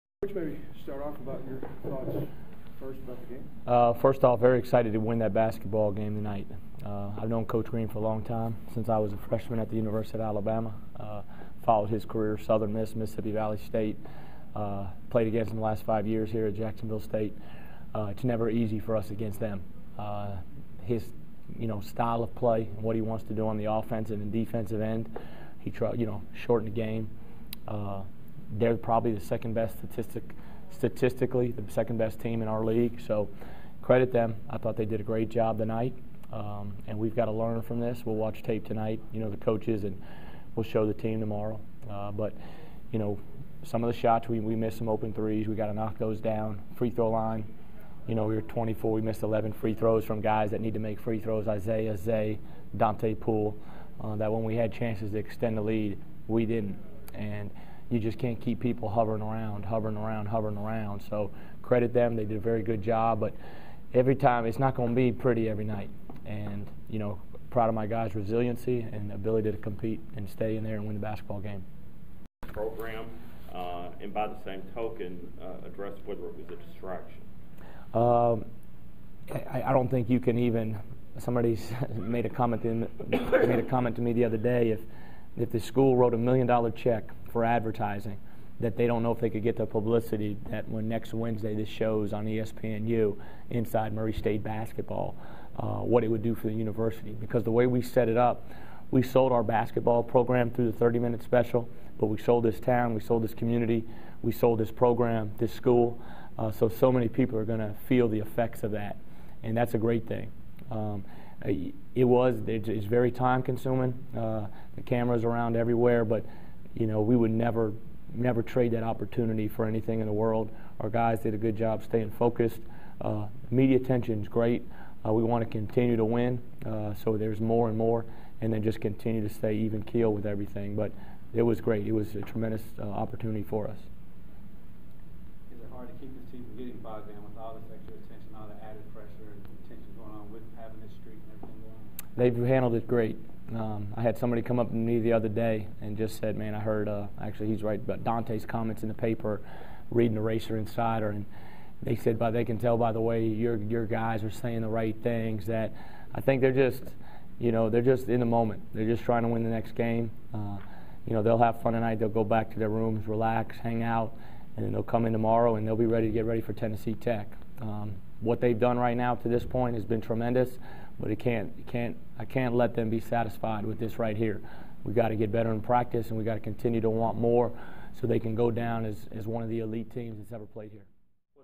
Steve Prohm-JSU Post
Murray State Head Coach Steve Prohm talks after the Racers defeated Jacksonville State 66-55 on Jan. 12, 2012.